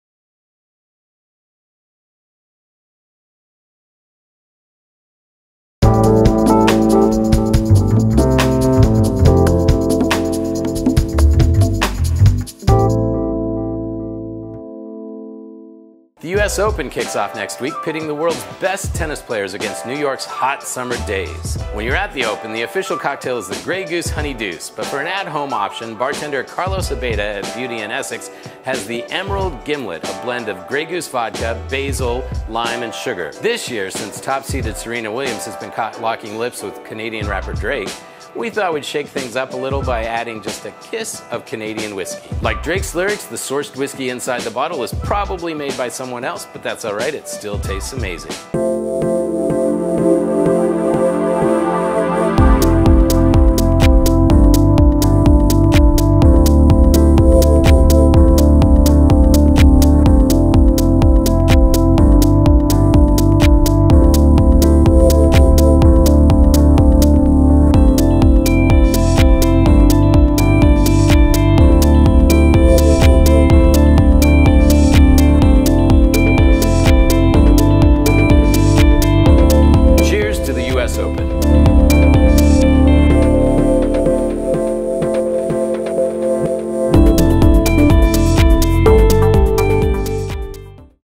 A Toast To The U.S. Open | Toast Of The Week
The U.S. Open kicked off this week, and to celebrate we recruited Carlos from Beauty & Essex in NYC to make a twist on the Grey Goose Honey Deuce (the official cocktail of the U.S. Open). Carlos used cantaloupe balls to reference tennis balls, and a basil simple syrup to give the whole thing a green color. And to give a little nod to Drake, who has been caught locking lips with Serena Williams lately, we added a "kiss" of Canadian whiskey.

The Emerald Gimlet (U.S. Open Edition)
¼ oz simple syrup
¼ oz basil simple syrup
1 ½ oz Grey Goose vodka
¾ oz lime juice
½ oz Pendleton whiskey
Cantaloupe balls for garnish